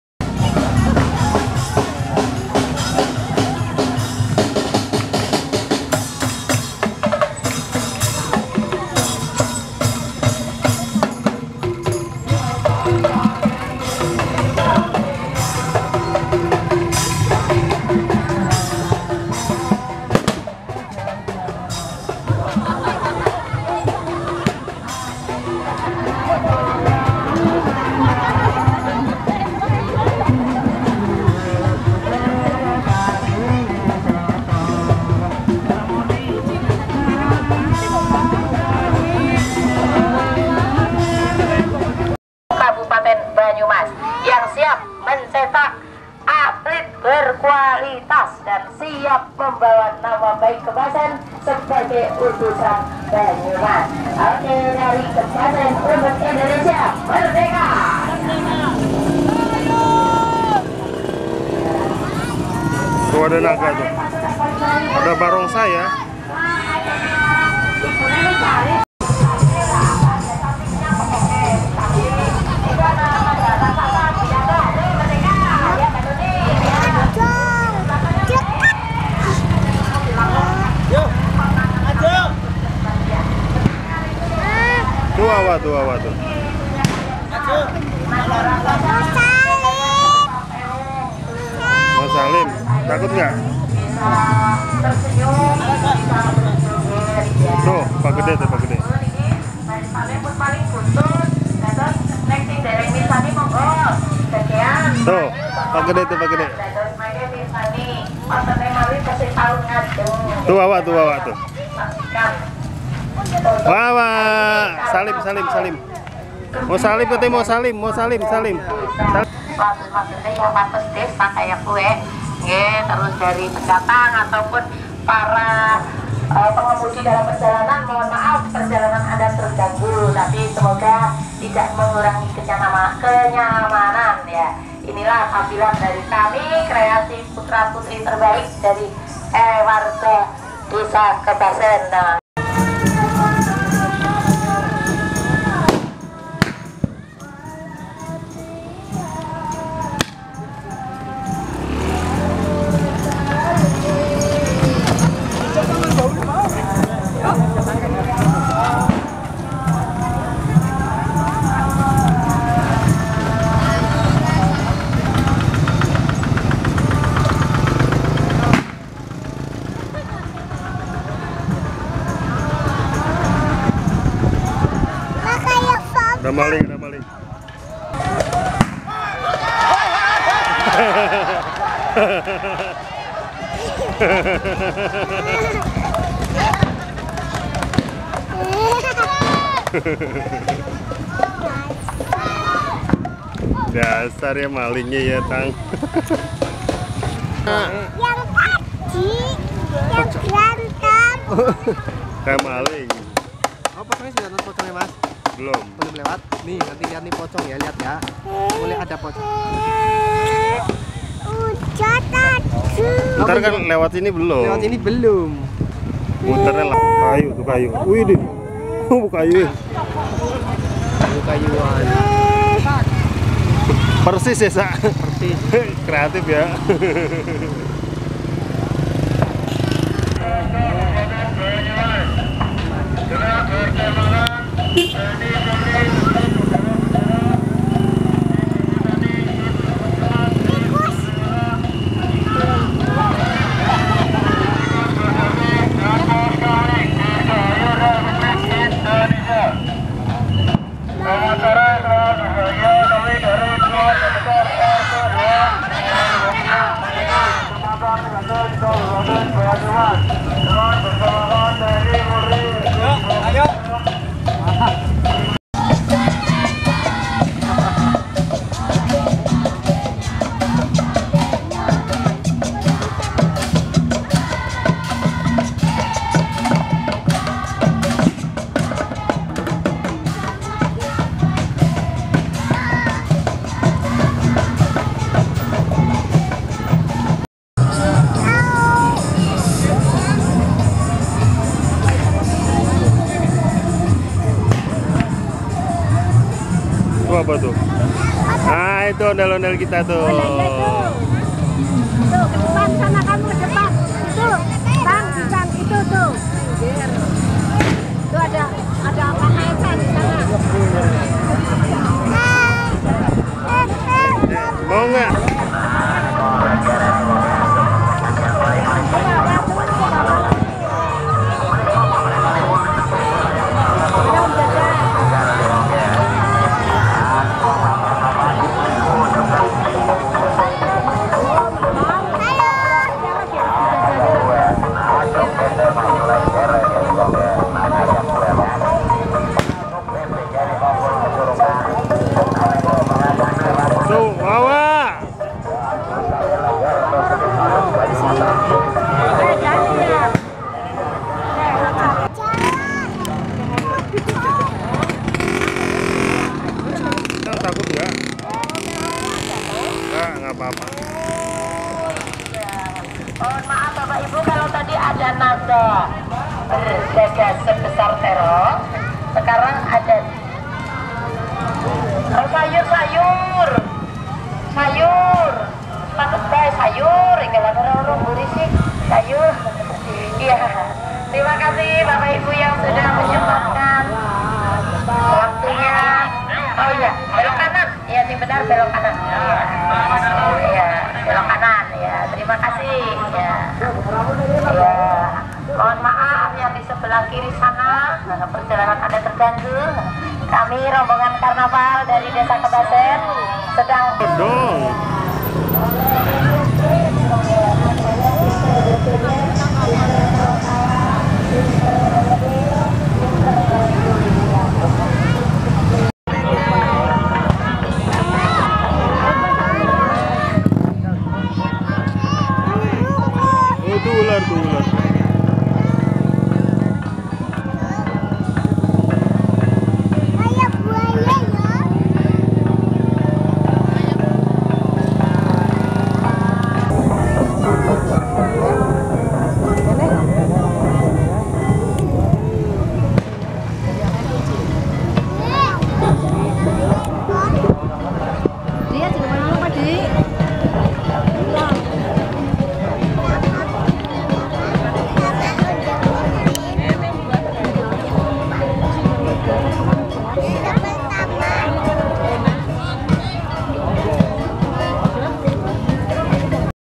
KARNAVAL DI BANYUMAS | 17AN 2015
Karnaval festival budaya rakyat th 2015, yang seperti ini sudah 3 tahun   sejak pandemi tidak pernah dilakukan rakyat indonesia. Semoga tahun ini 17an kembali meriah.